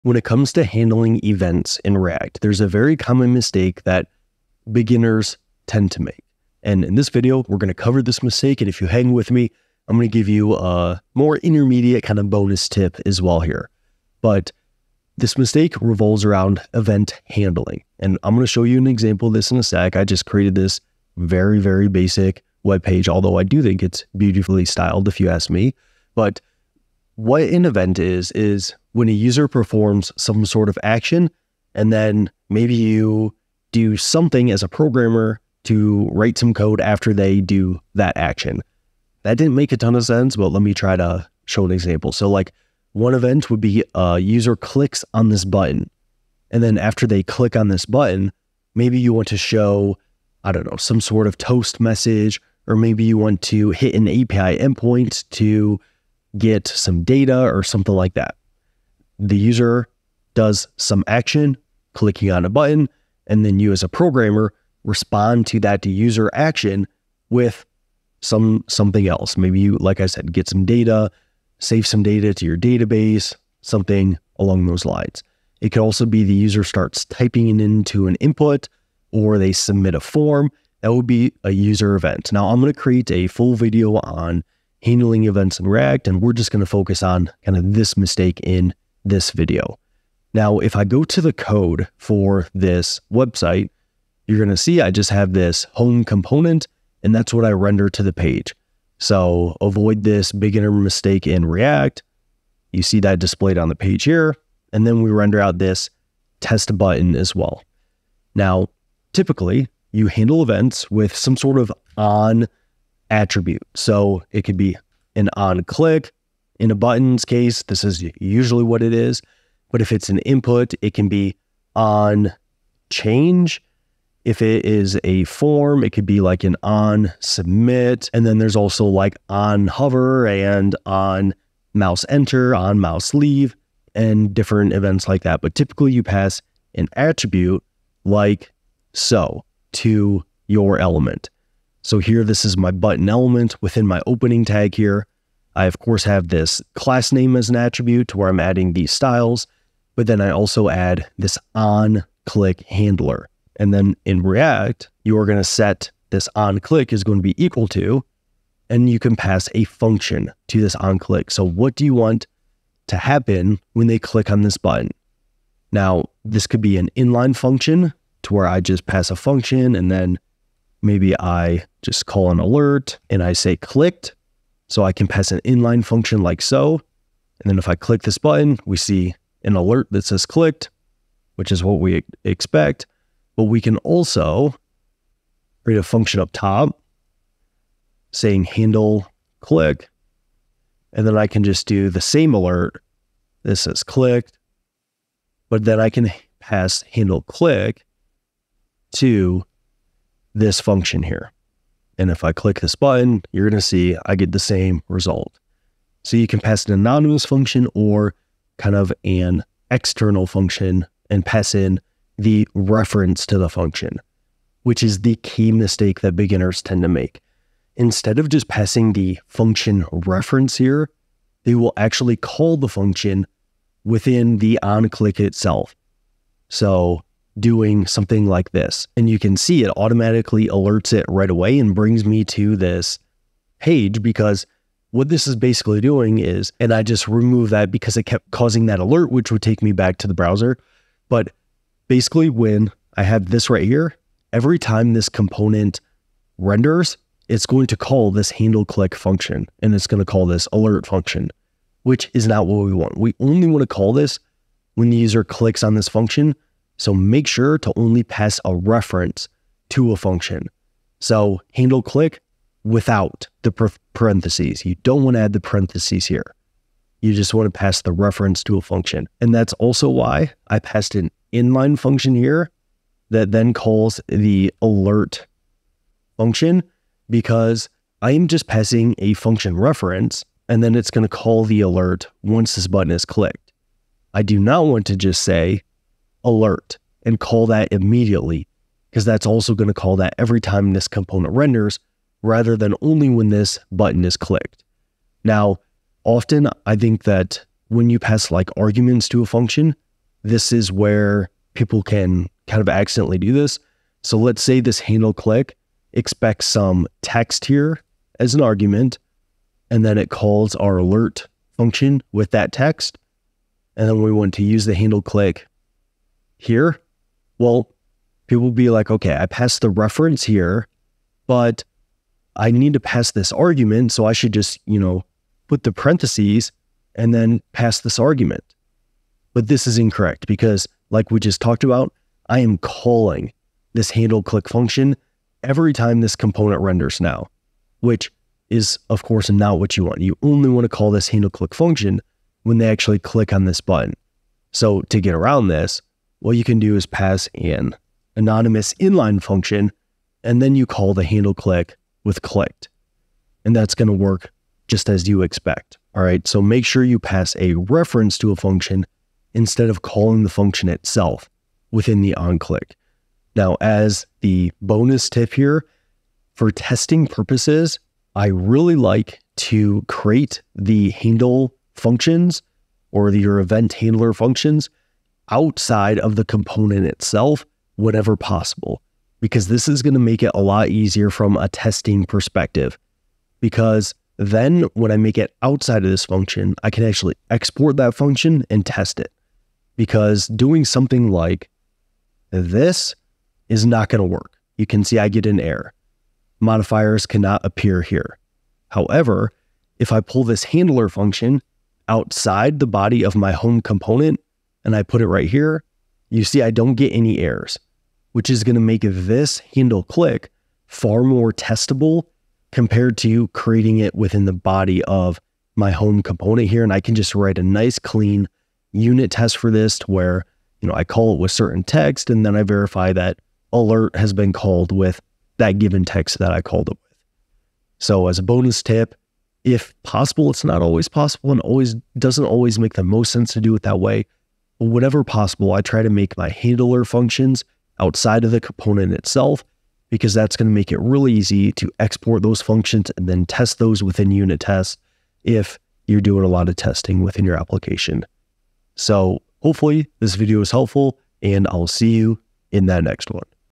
Avoid This Common React Mistake Newbies Make
Topics Covered:

What is an event?
How are event handlers typically passed?
Calling a function rather than passing a reference to a function
But what if you need to pass arguments?
When possible, I like creating functions outside of the component so I can test them a bit easier via exports